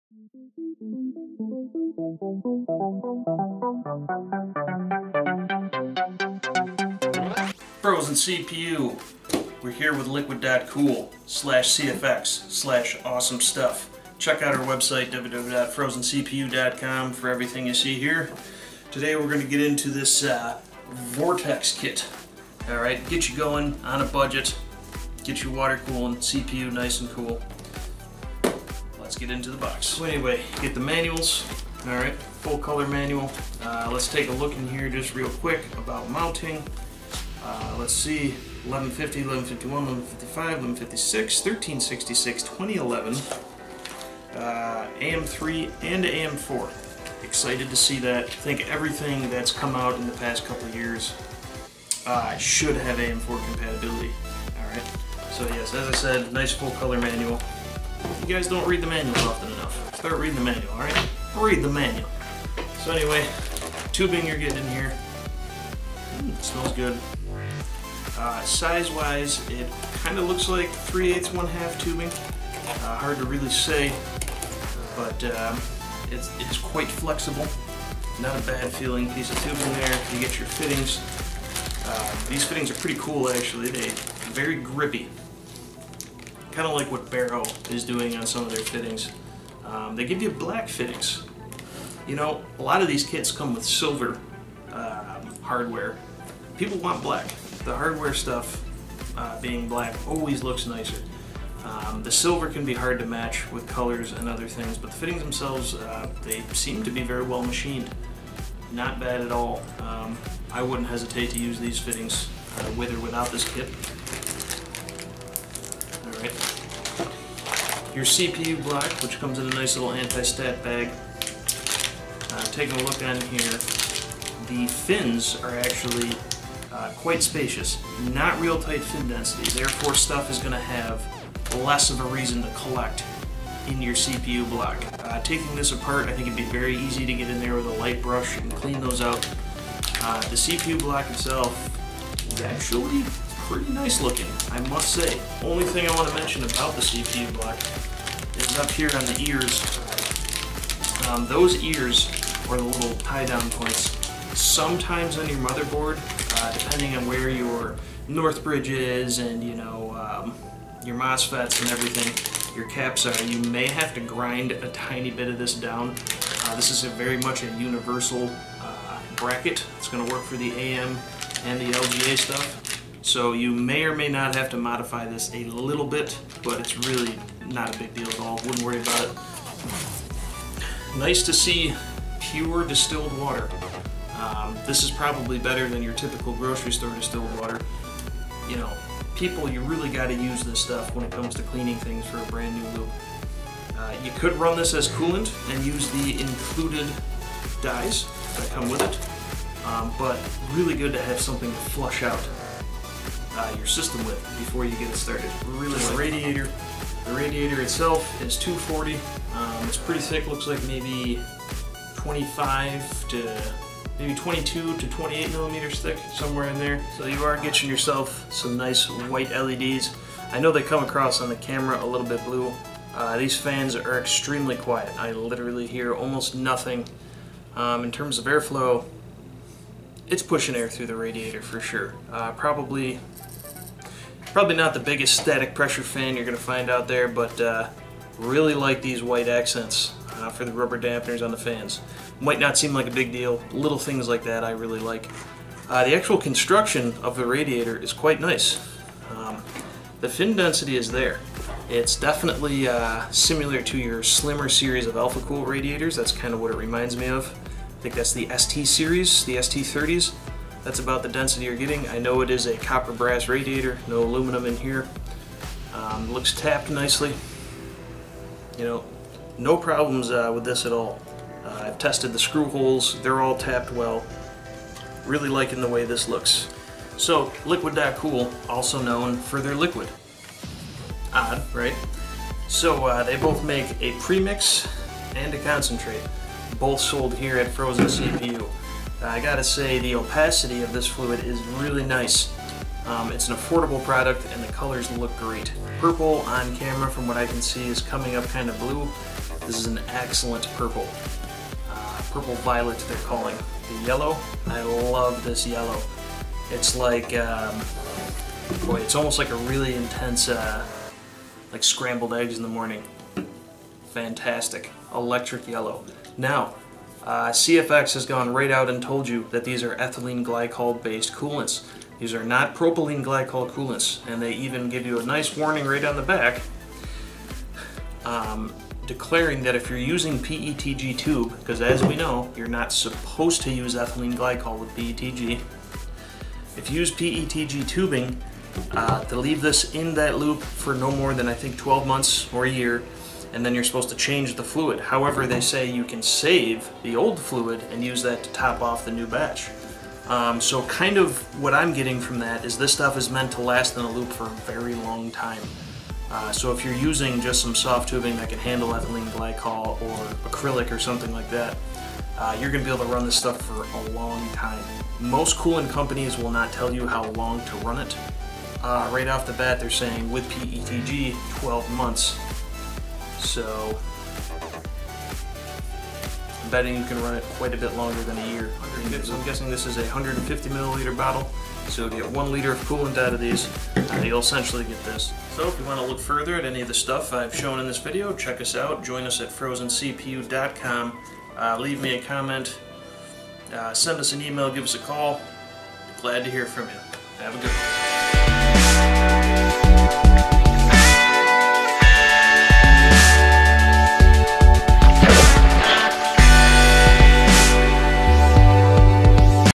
Vortex One Advanced DIY 240mm Water Cooling Kit
The new Liquid.cool Strike ONE CPU block, designed for a High Flow setup while offering enthusiast performance, design and appearance.

Take your PC’s cooling performance to the next level and allow your components to be pushed further using this custom 240mm DIY cooling kit from Liquid.cool. The kit itself has everything you may need to create a custom liquid cooling set up to meet your needs. The 240mm copper radiator can disperse heat very efficiently and the included fans keep the radiator and your CPU cool under heavy loads. Custom liquid cooling can give you much better cooling performance compared to all in one cooling systems and traditional air coolers as you can make specific tweaks to your system to have it perform better.

Liquid.cool Vortex One Advanced DIY 240mm Water Cooling Kit

Liquid.cool CFX Pre Mix Opaque Performance Coolant - 1lt